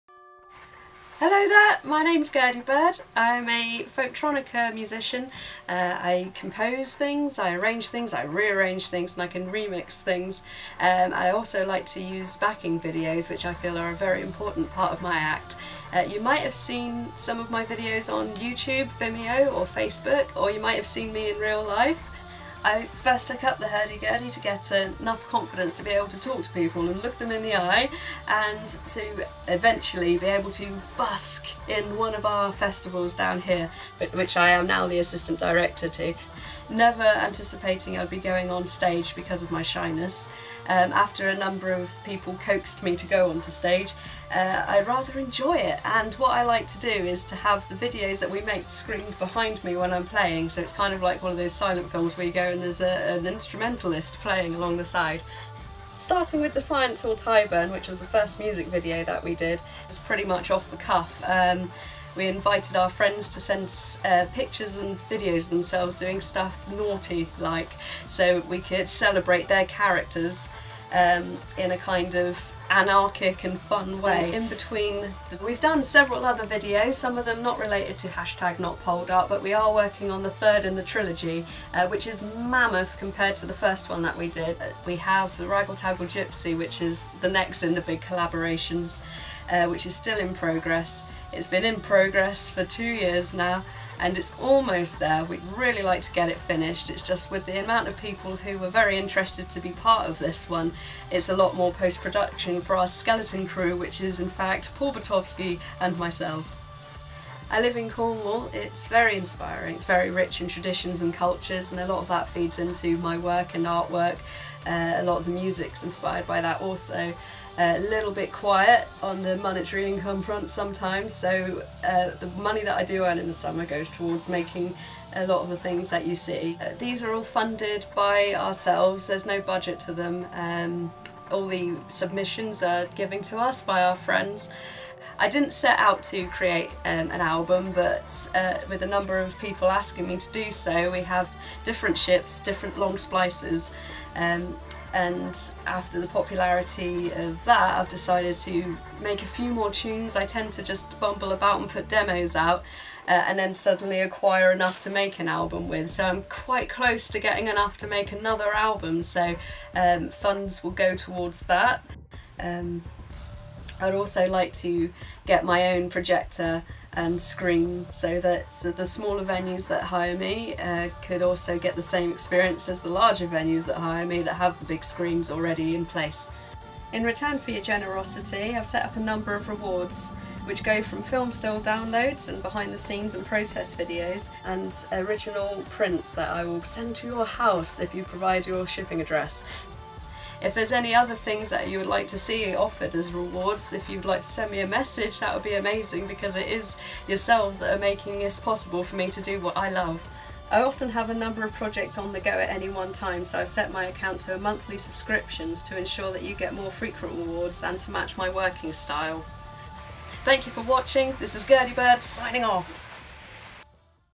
GurdyBird's Patreon Pitch
GurdyBird is now on Patreon. By becoming a patron and supporting her on a monthly subscription, you can receive a number of rewards from tier of your choosing and enable her to spend more time creating.

What your patronage will go towards
GurdyBird lives in Cornwall, which is beautiful, very rich in culture and history, it inspires an enormous of her work, but when winter comes it does get rather quiet on the monetary income front, and she currently make these videos that stream for free with no budget at all, but what she and her friends do have in vast quantity is a lot of creativity, enthusiasm, and s a keen work ethic.

While the videos and music is a constant activity she is working on, she is also a trained illustrator and hand prints a lot of artwork, relating to some of the subjects in the videos, for example, Montol, just one of the festivals she volunteers her time and skills to as part of The Cornish Culture Association. She also has digital prints created of some of her artworks and badges also, and GurdyBird has written some stories which she is hoping to publish as booklets like the chapbooks of the 18th century.